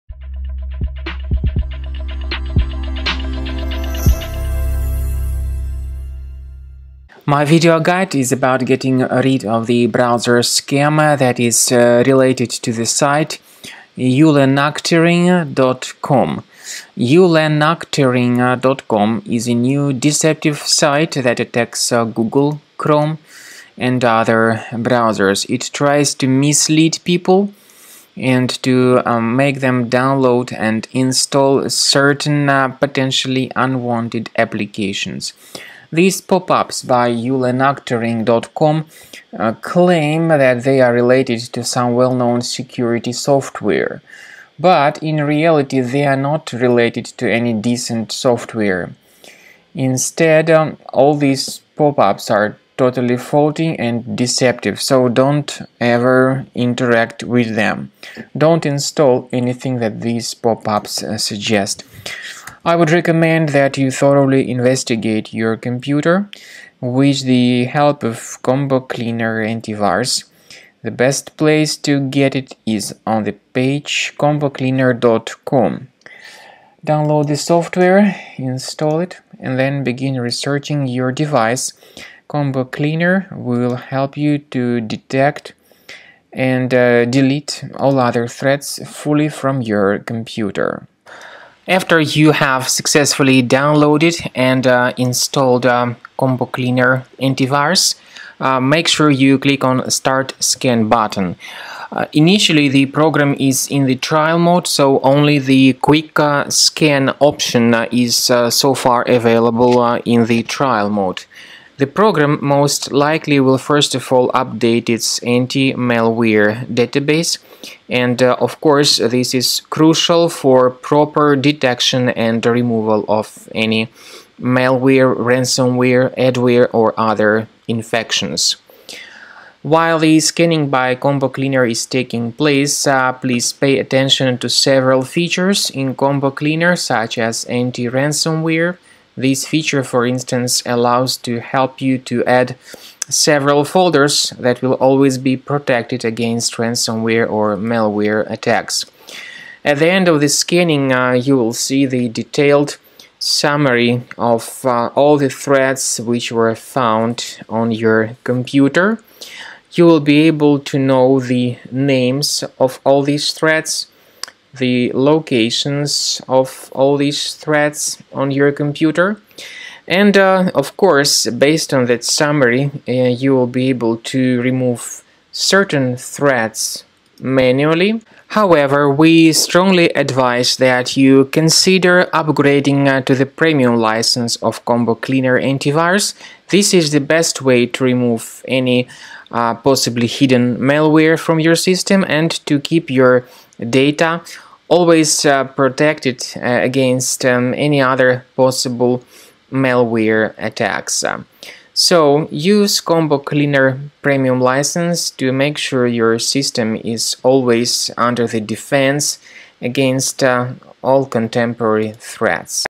Ulanucterting.com online scam (removal guide).
Ulanucterting.com is a scary pop-up that attacks Google Chrome and other browsers. It tries to make you scared about the condition of your computer and to make you download and install some potentially unwanted applications. Get rid of them with the help of Combo Cleaner Antivirus via the official or, alternatively, follow the manual instructions provided in the video tutorial on how to fix your browser.